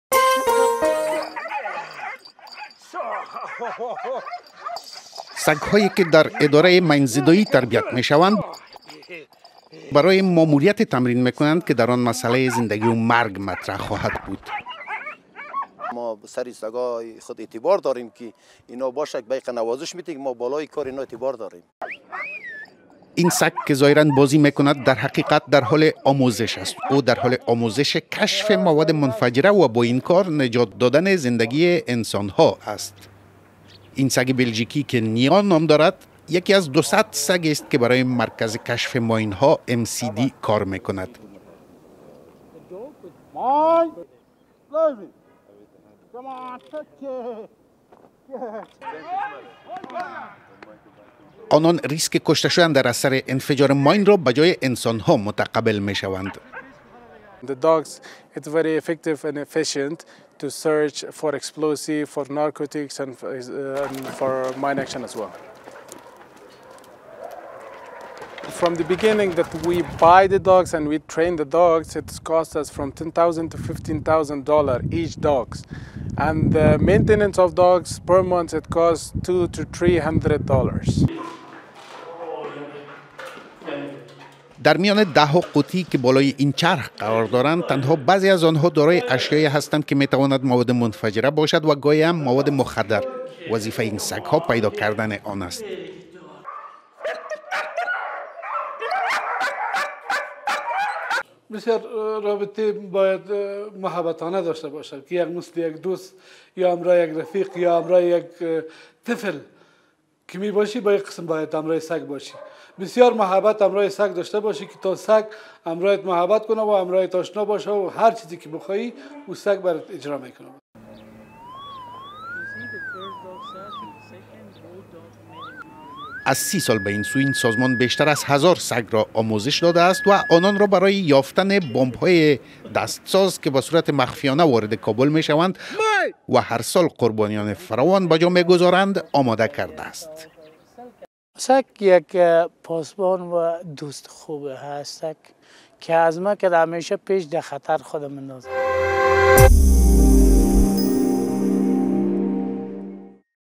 سگ های ماین یاب در افغانستان
سگ های که در ادارۀ مین زدایی تربیت میشوند،
برای ماموریتی تمرین میکنند که در آن مسئلۀ زندگی مرگ مطرح خواهد بود.
آنان ریسک کشته شدن در اثر انفجار ماین را به جای انسان ها متقبل میشوند.